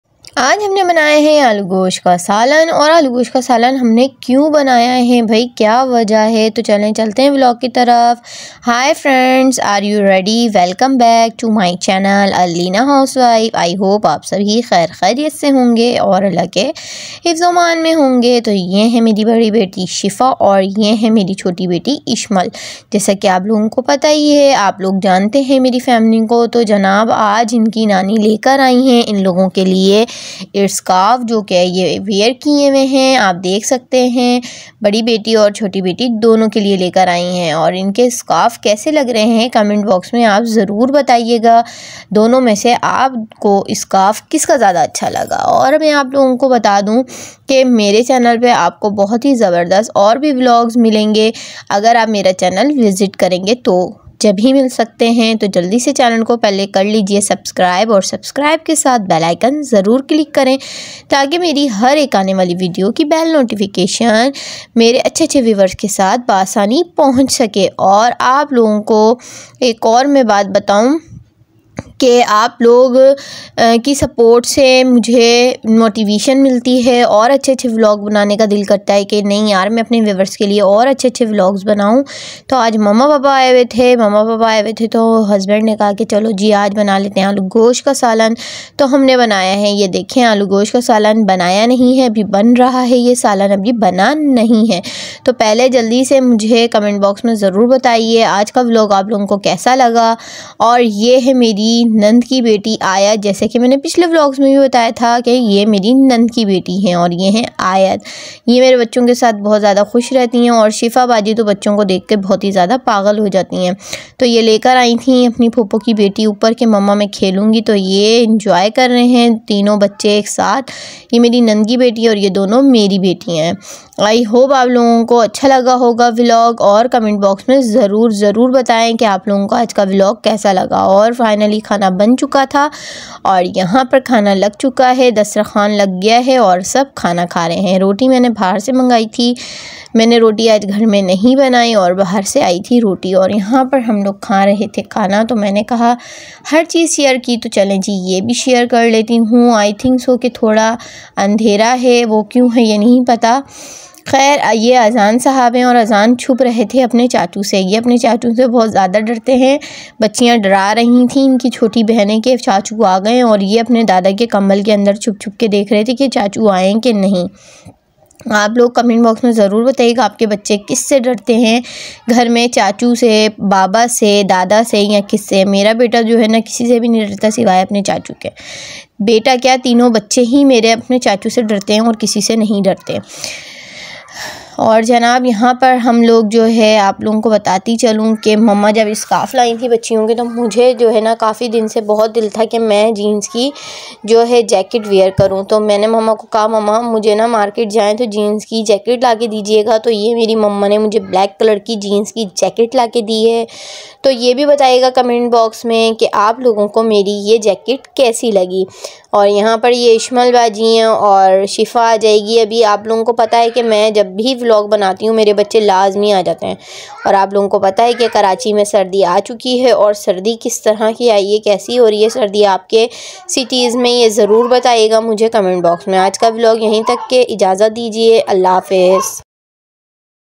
Sardiyan Aa Gayi || Ye Jacket Khnse Ai || Mama Papa Achanak Q Ai
Hi friend aap sab theek Honge to aaj main vlog Lekar I hun bahut hi zabardast to wo vlog hai kya mama Baba Aaye Hain Hamare ghar per Kyon Aaye Hain yah aap log Dekhkar hi pata chal sakta hai aap Logon Ko to jaldi se vlog ko full watch Kijiye aur subscribe karna mat bhuliyega thanks for Watching❤️❤️❤️